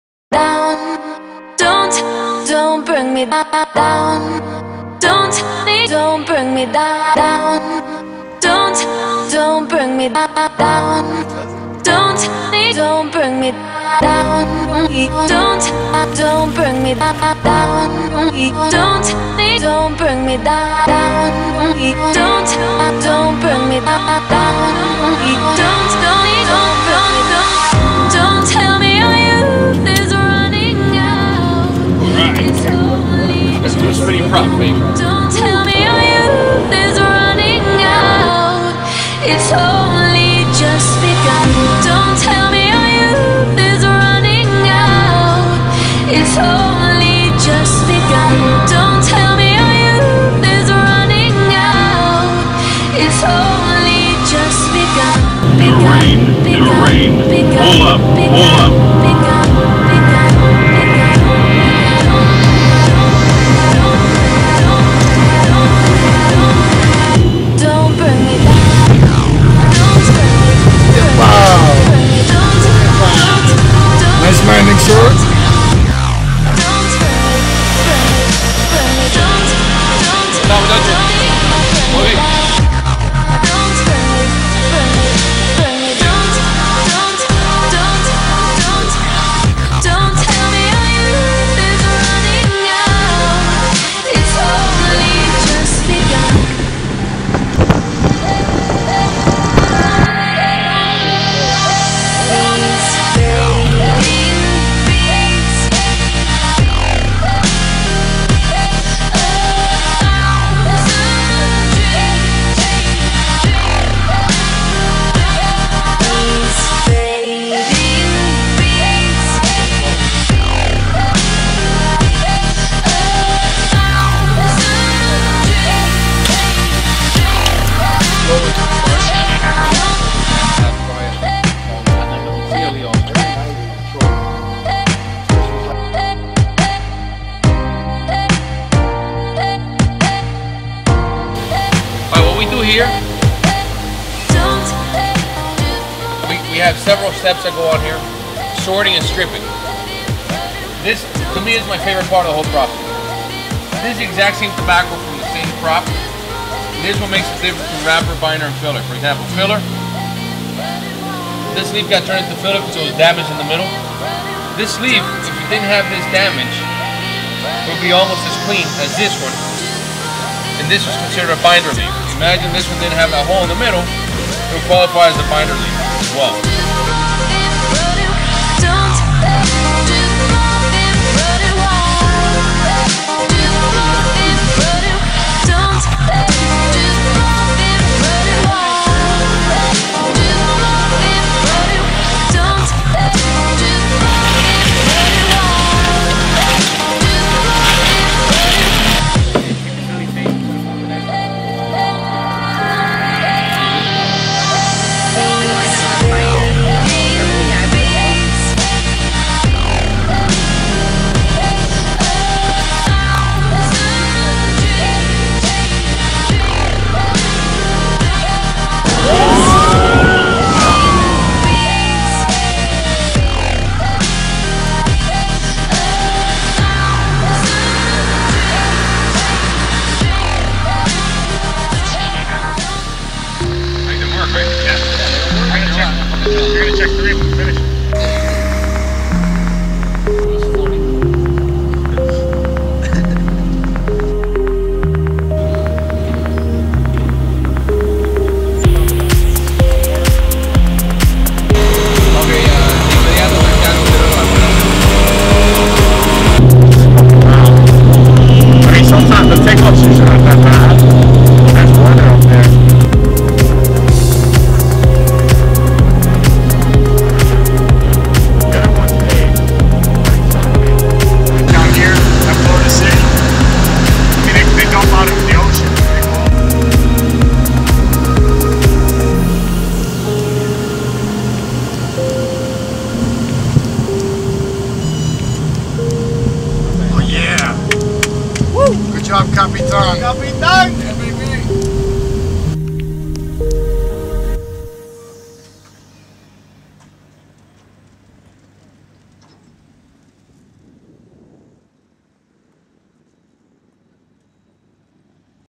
CLE Cigar Factory in Honduras
Christian Eiroa visits his CLE Cigar Factory in Honduras.